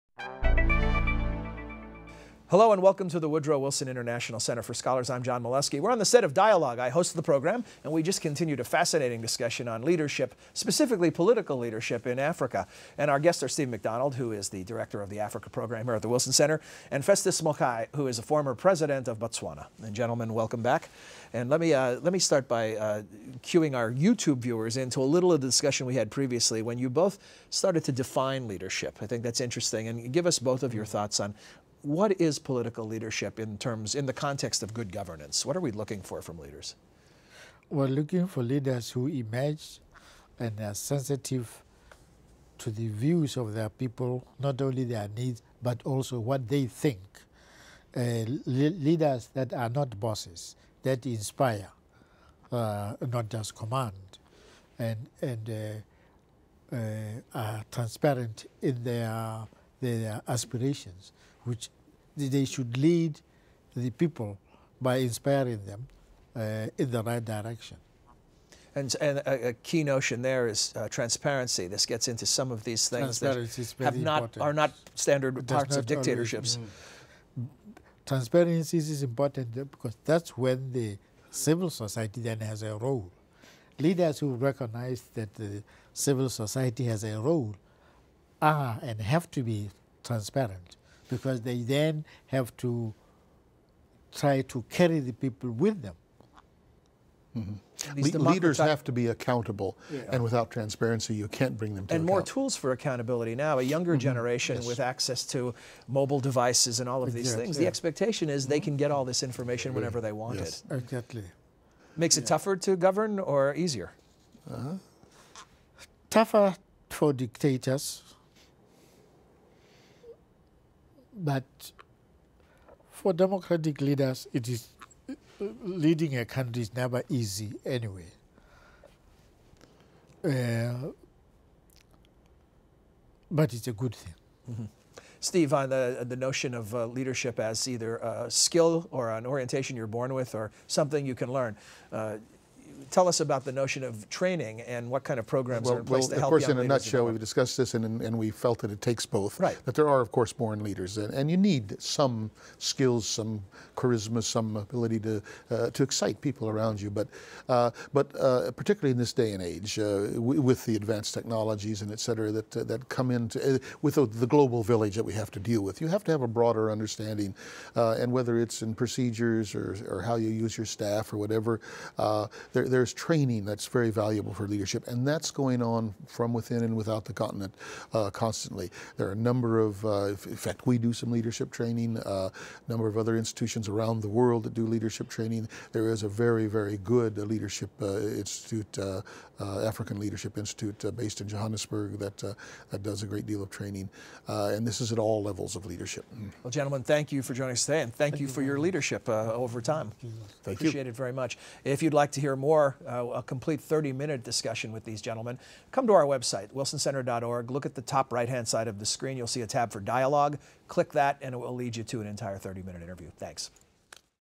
Leadership in Africa
This week on dialogue host John Milewski discusses the critical role of political leadership in Africa with Festus Mogae and Steve McDonald. Festus Mogae is a former President of the Republic of Botswana (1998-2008) and a former Wilson Center Public Policy Scholar. He leads the organization, Champions for an HIV Free Generation in Africa, and chairs the Council of Elders. Steven McDonald is Consulting Director of the Wilson Center's Africa Program and of the project on Leadership and Building State Capacity.